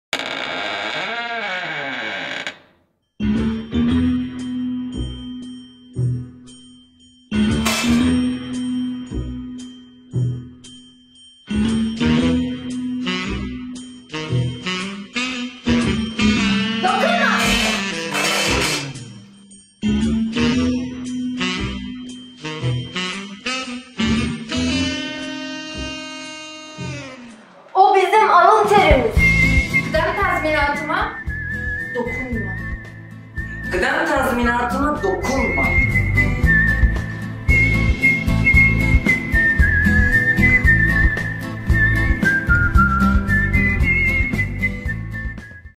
Kıdem tazminatıma dokunma
DİSK’e bağlı Gıda-İş Sendikası hazırladığı video ile ‘kıdem tazminatıma dokunma’ diyor.
“Kıdem tazminatı iş güvencemiz, alın terimiz ve çocuklarımızın geleceğidir! Kıdem tazminatı hakkımızı elimizden almaya çalışanlara karşı birleşelim, örgütlenelim ve mücadele edelim!”